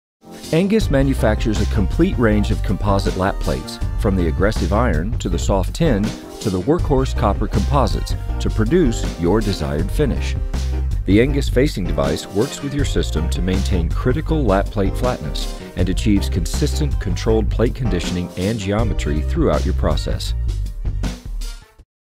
Engis Complete Range Of Composite Lap Plates
One of the best tools to maximize the efficiency of your lapping operation is the proper selection of a lapping plate. Engis designs and manufactures a complete range of composite lap plates for every application. Our composite lapping plates allow for efficient charging of the diamond abrasive particles and can be fine tuned either for aggressive stock removal, or for achieving fine finishes and precise flatness.   Visit our web site for more information To watch our complete video visit this